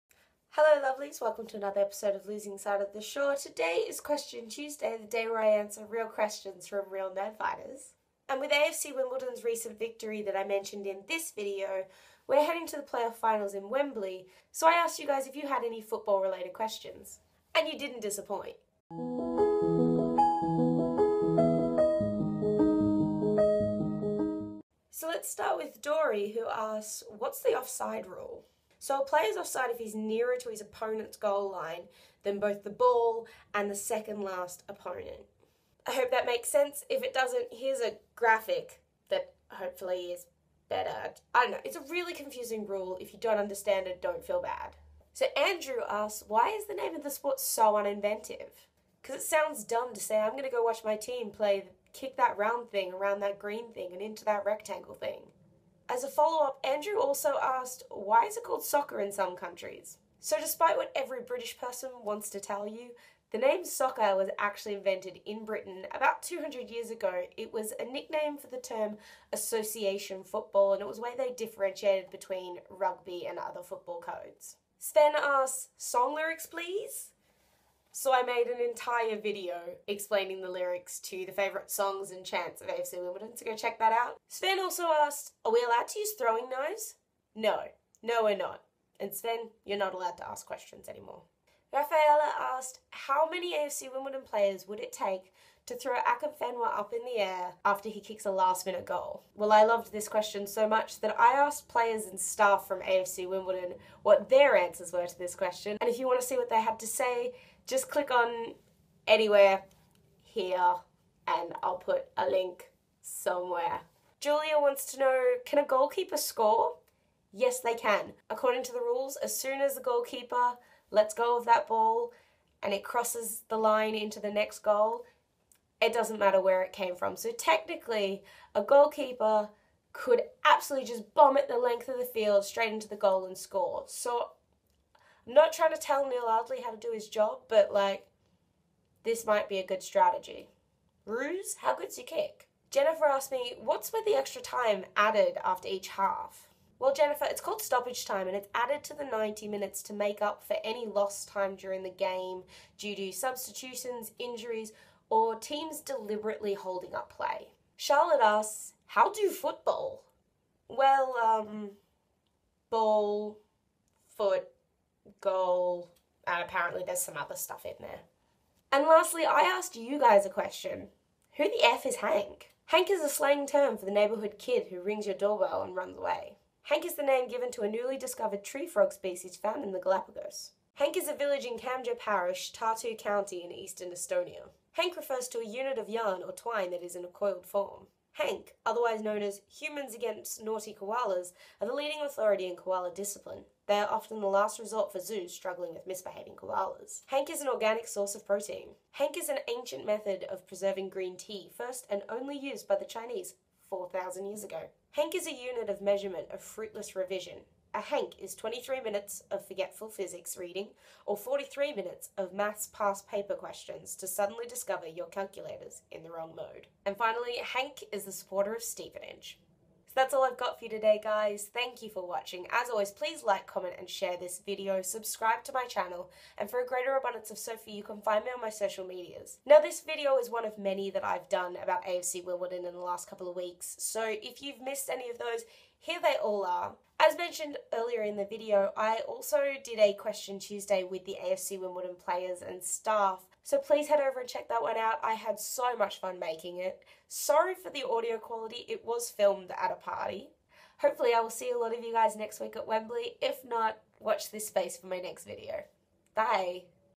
Question Tuesday: Explaining Football to Nerdfighters [LSotS Ep47]
In the lead up to the league two play-off final at Wembley on Monday, I asked the Nerdfighters if they had any questions that like me to answer to get them ready